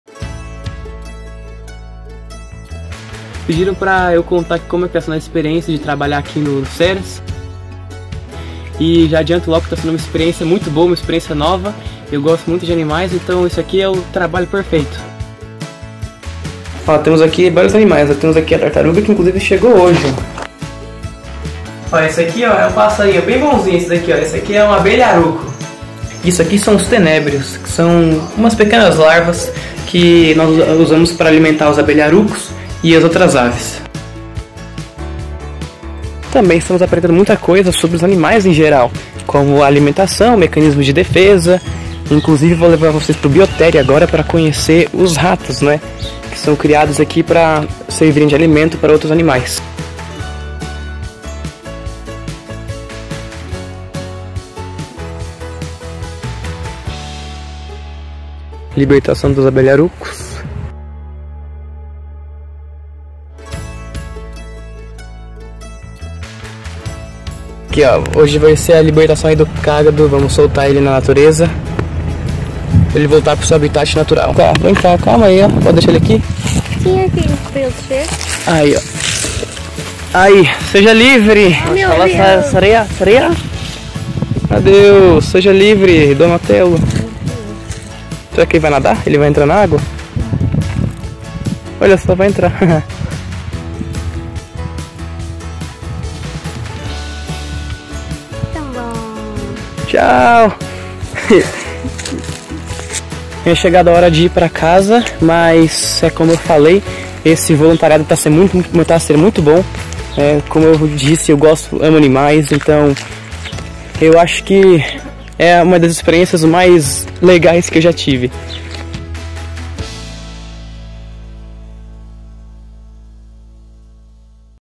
Testemunho DIJ 2023 - Nicolas Pacanaro
Dia Internacional da Juventude 🌍🙋💁‍♀️
«Competências verdes para a juventude: Rumo a um mundo sustentável»🌳🌳

Neste dia simbólico, deixamos o testemunho Nicolas Pacanaro,💪 👏 voluntários do Programa Voluntariado Jovem para a natureza e Florestas, no CERAS, em Castelo Branco.
Obrigado, a tod@s! 🤜🤛

A tod@s os/as jovens, um excelente Dia Internacionall da Juventude!